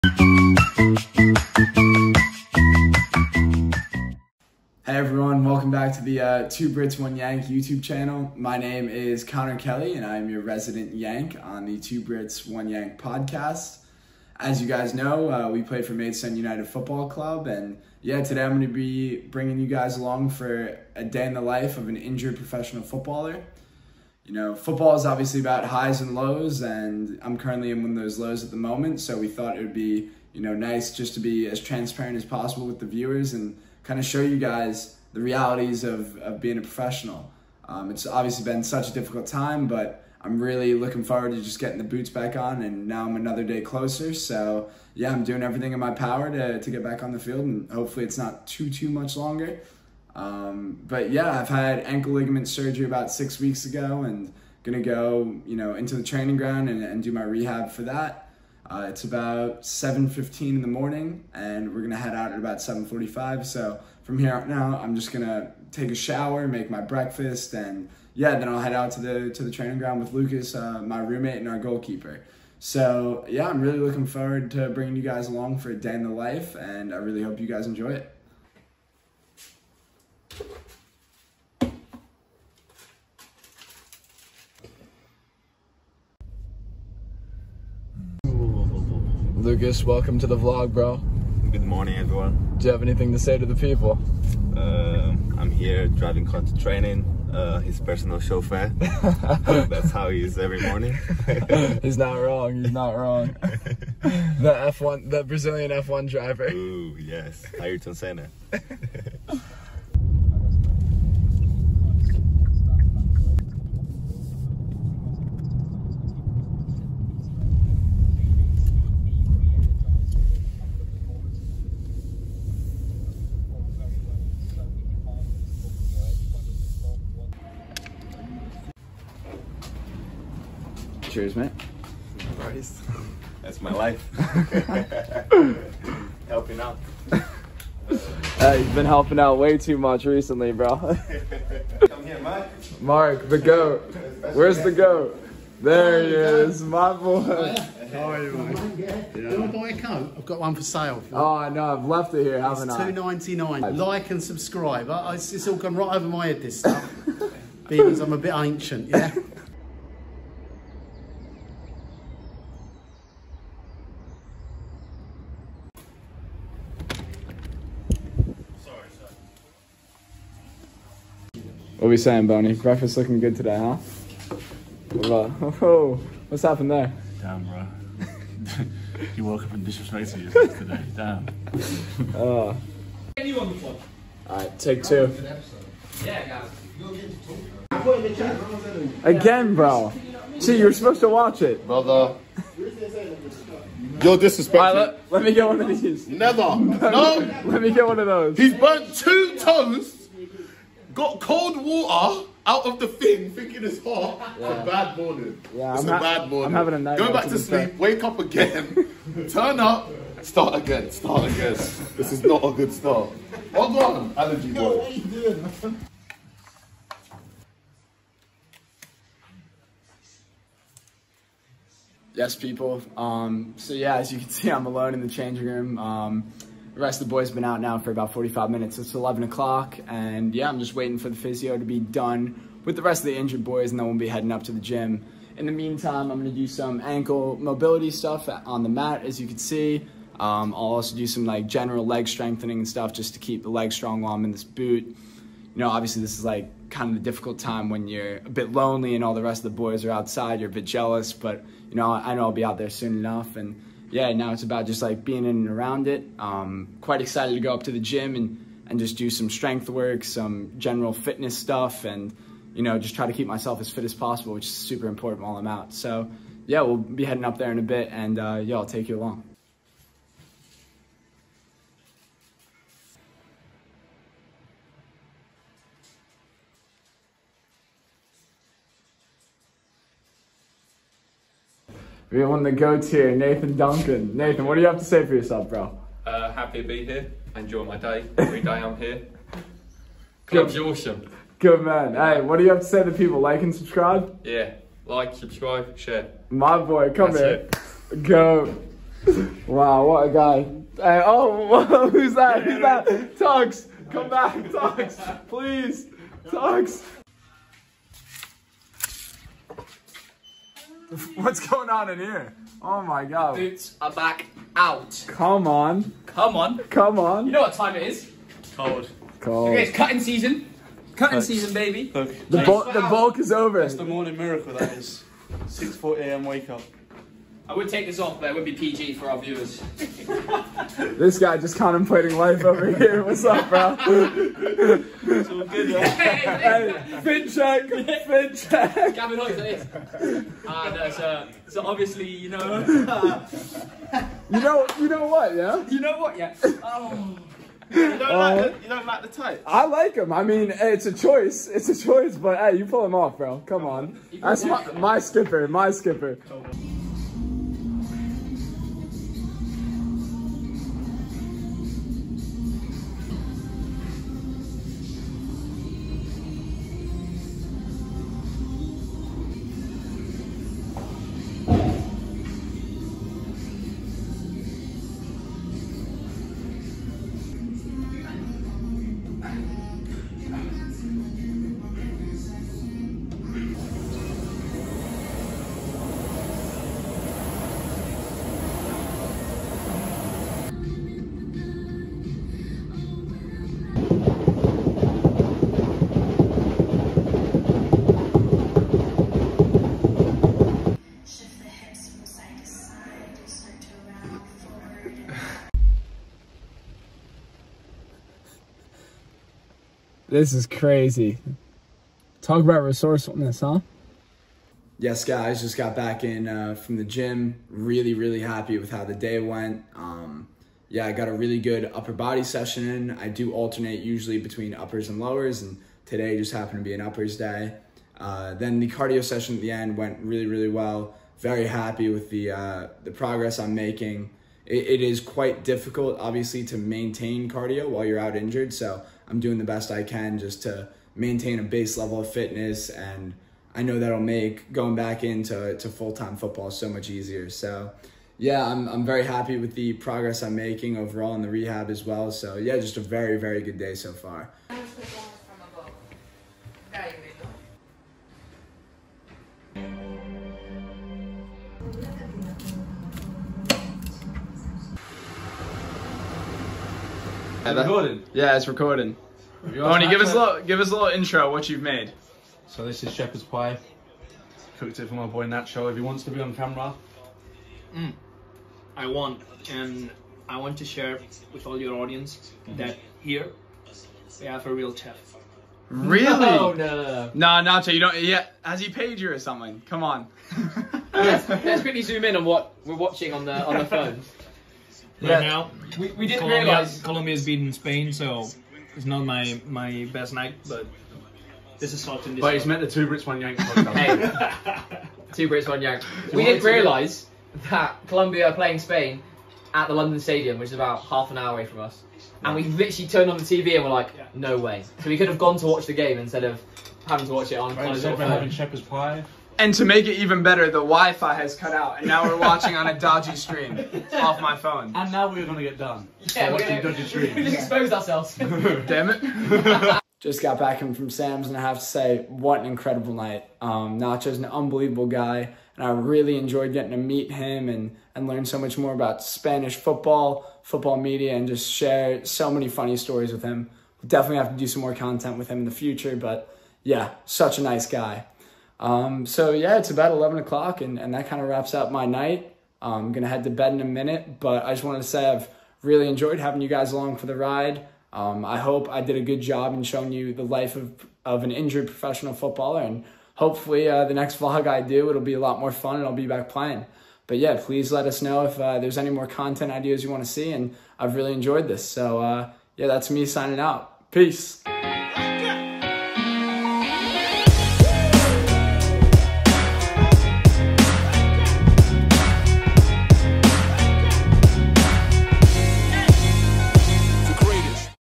Conor Kelly DAY IN THE LIFE : Injured Professional Footballer Edition FT. NACHO GONZALEZ
Maidstone United player Conor Kelly takes viewers along with him for a day in the life, featuring La Media Inglesa journalist, Nacho Gonzalez.